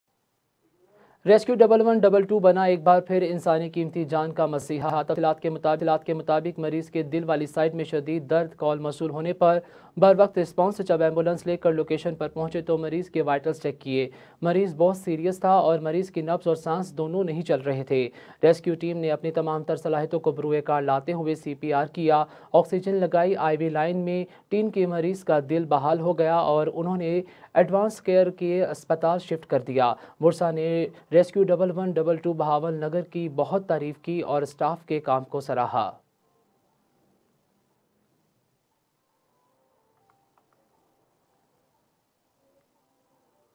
"1122 Emerges as the Lifesaver in Bahawalnagar: Voice News Report"
Voice News presents a report highlighting the crucial role of 1122 emergency services as the savior of precious lives in Bahawalnagar. Explore the recent incidents, response times, and impact of 1122 services on the community's safety and well-being. Stay informed with our coverage as we delve into the invaluable contributions of emergency responders in Bahawalnagar.

What recent incidents in Bahawalnagar have underscored the importance of 1122 emergency services, according to Voice News?
How has 1122 once again proven to be the lifesaver for residents facing emergencies in Bahawalnagar?
What are the response times and protocols followed by 1122 responders during critical situations?
How does the community perceive the role of 1122 services in ensuring public safety and addressing emergencies?
In what ways does Voice News highlight the dedication and effectiveness of 1122 personnel in Bahawalnagar?